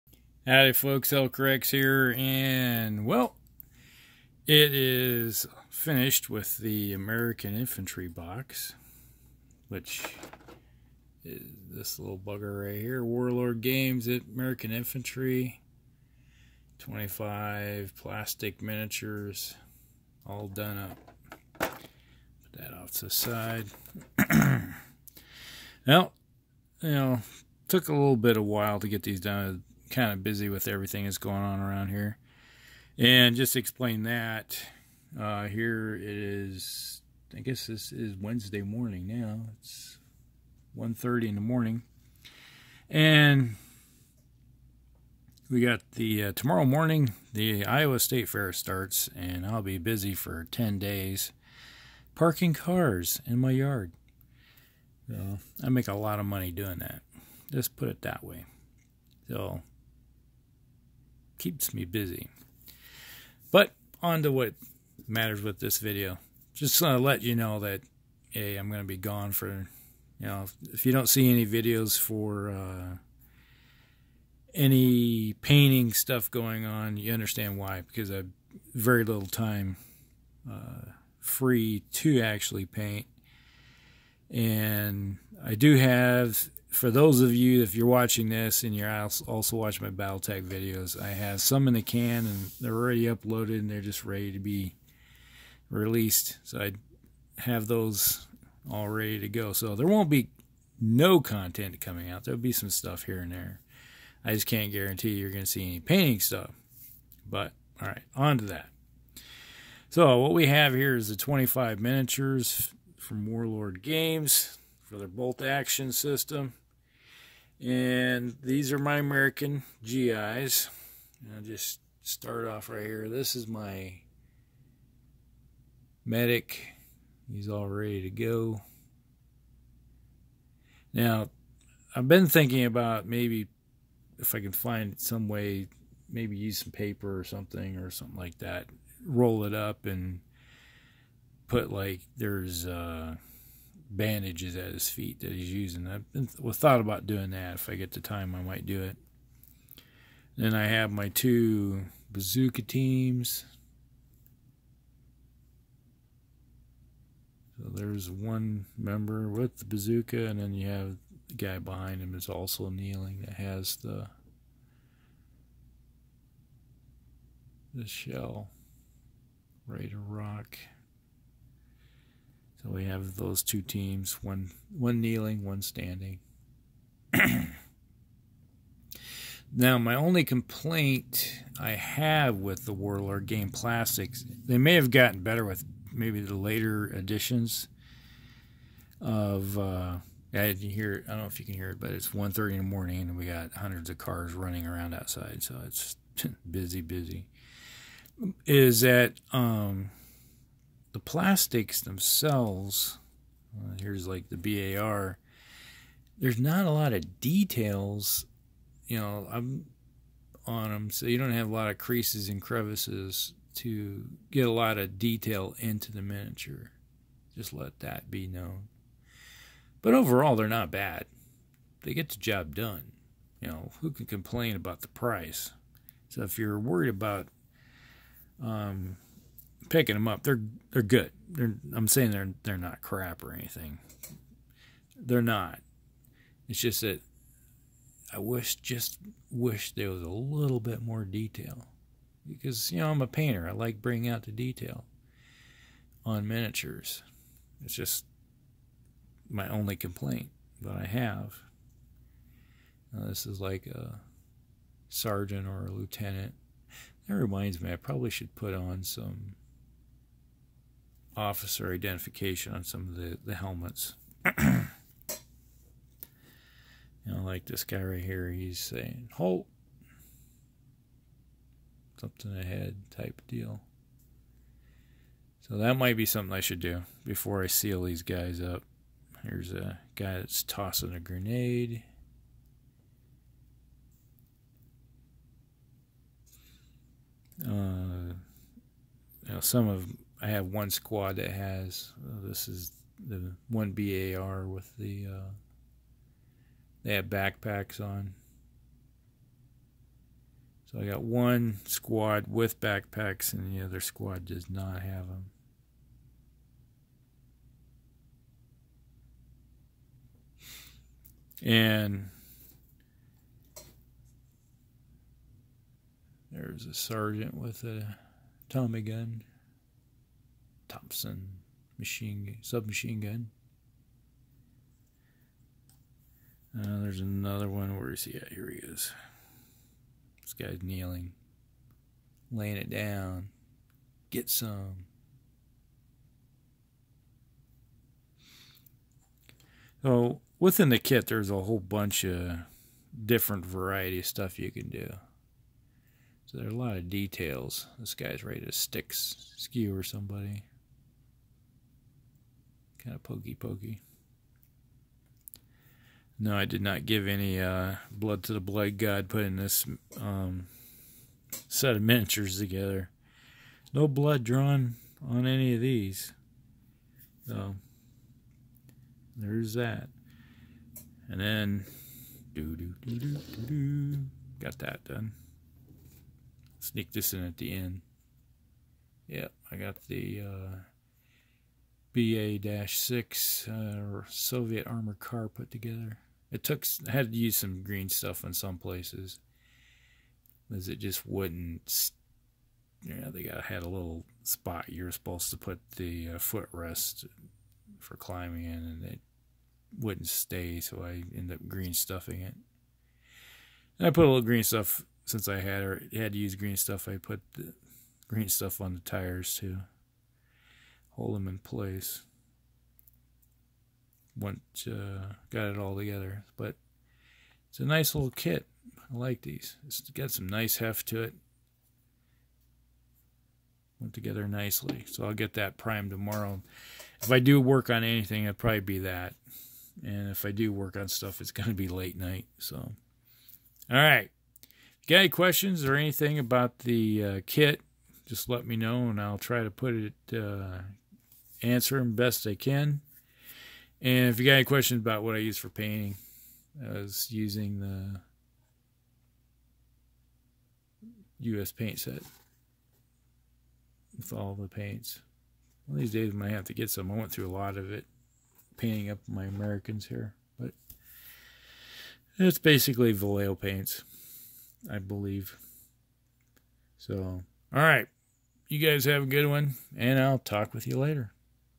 Warlord Games Bolt Action WWII American Army Infantry Box
I have finished painting up my 28mm American Infantry box for Bolt Action.